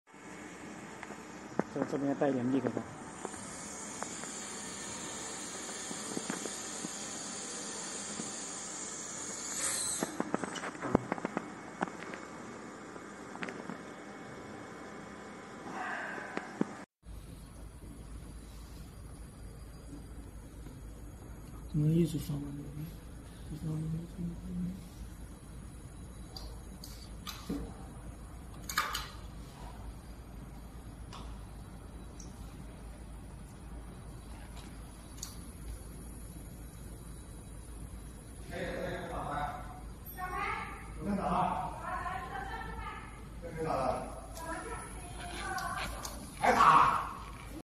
Adhesive silicone gasket strip laminating machine ES -020
1, Good lamination quality with adjustable fixtures and customised laminating rollers ,air powered laminating force,well controlled dipping and brushing amount silicone primer

2, Primer brushing  on silicone strip and lamination of silicone strip with double side tapes in one machine

3, High production efficiency ,save labour to  brush primer on silicone strips and then press join silicone strip with double side tapes manually